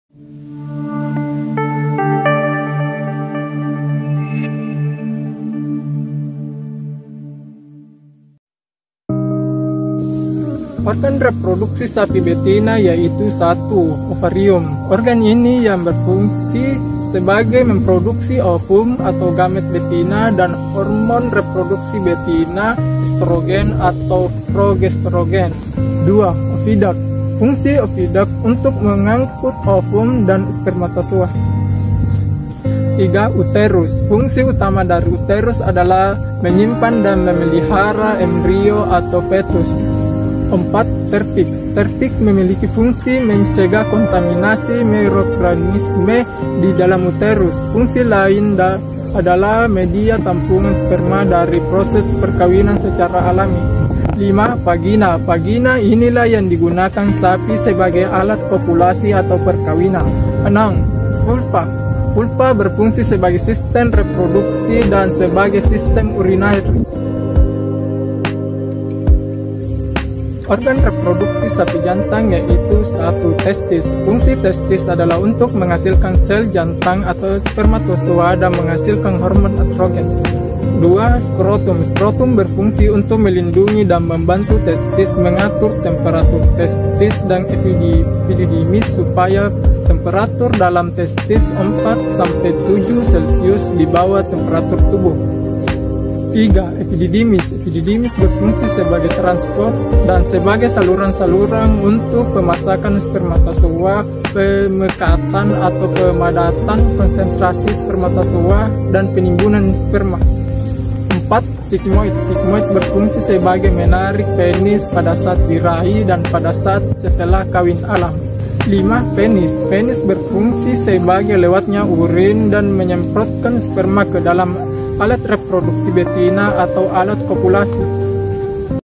Organ Reproduksi Ternak Sapi Jantan & Betina oleh Hasanuddin
Sebagai media pembelajaran agar mahasiswa dapat memahami dan menjelaskan pengertian dan fungsi dari organ reproduksi ternak sapi jantan dan betina. Disampaikan oleh Mahasiswa Peternakan Polbangtan Gowa Kelas I E Prodi Penyuluhan dan Kesejahteraan Hewan (Hasanuddin)

Mantap sekali, tetap semangat belajar untuk mengejar cita-cita.